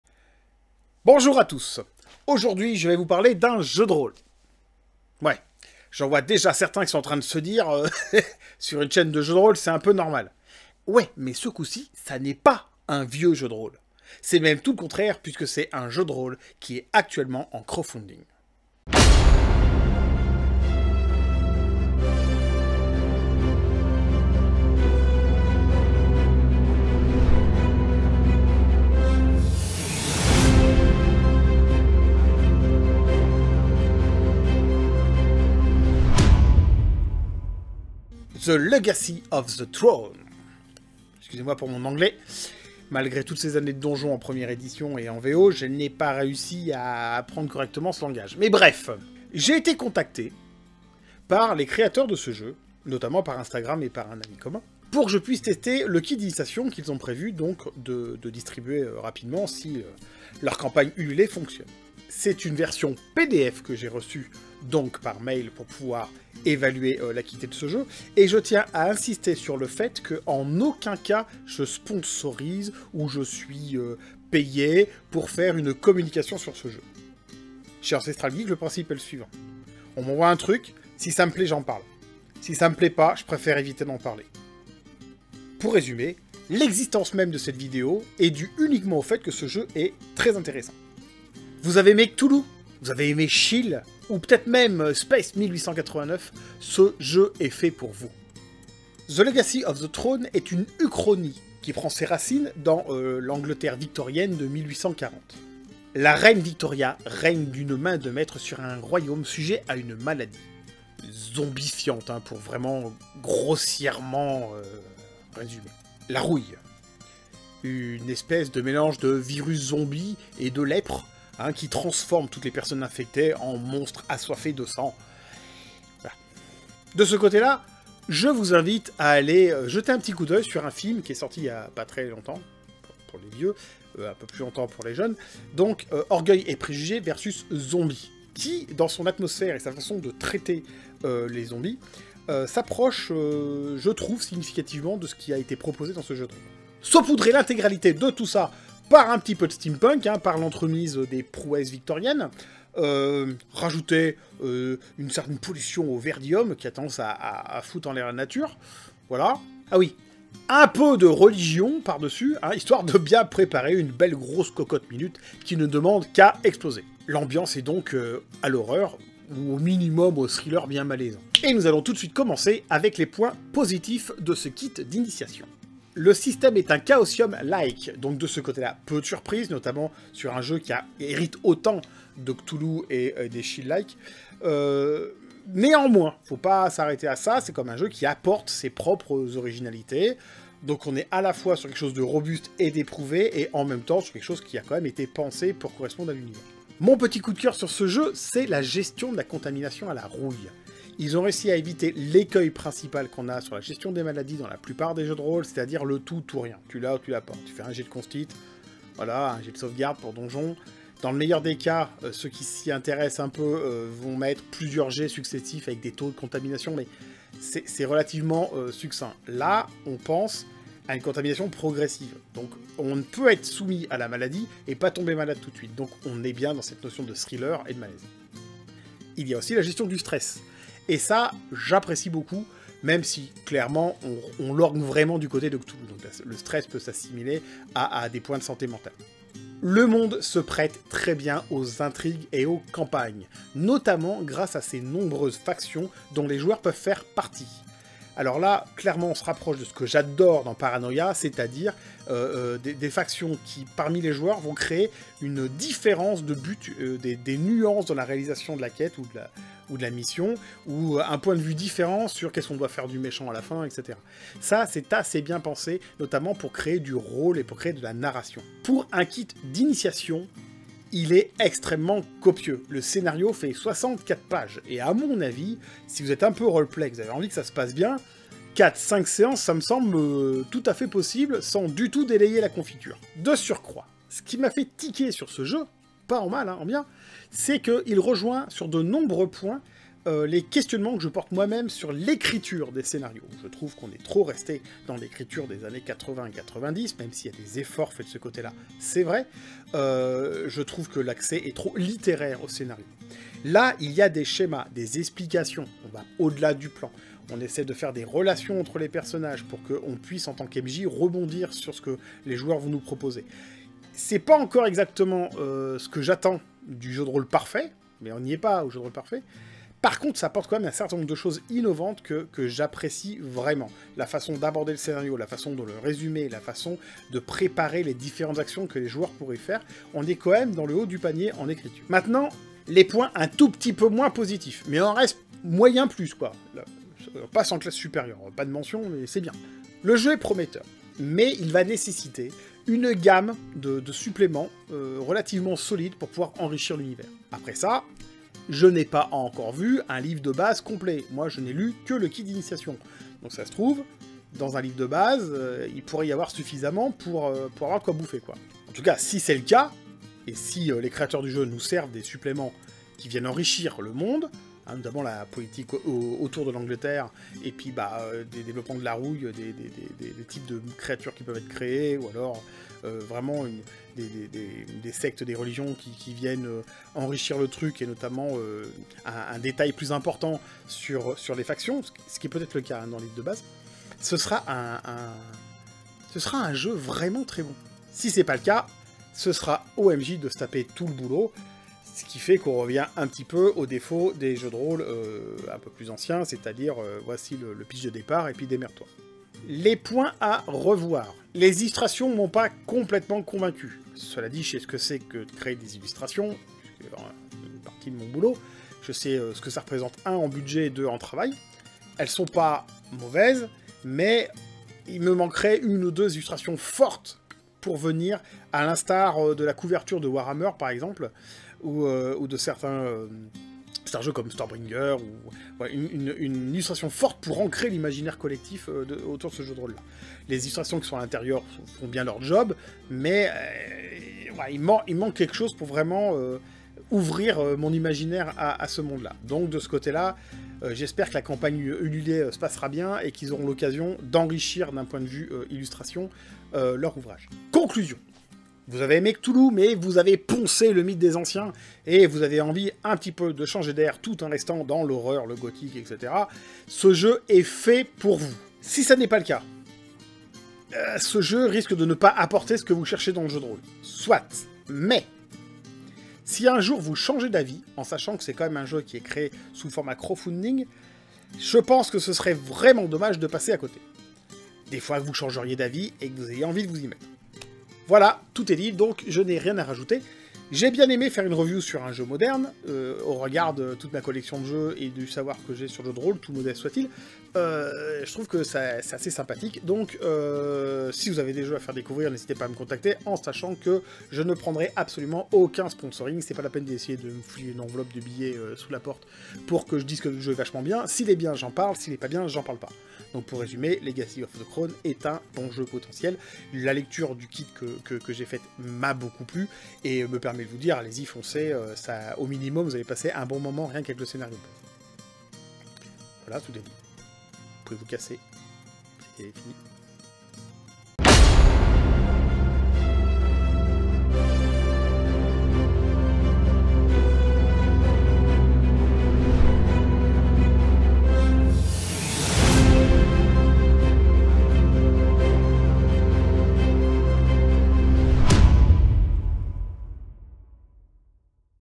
[ TEST JDR ] - The Legacy of the Crown - Ancestral Geek Review01
Review du  Jeu de Rôle - The Legacy of the Crown - actuellement en Crowfunding sur ulule :

7ème Vidéo de l'Ancestral Geek qui chronique ses jeunes années de rôliste .
mais également des nouveautés en Jeu de Rôle !!!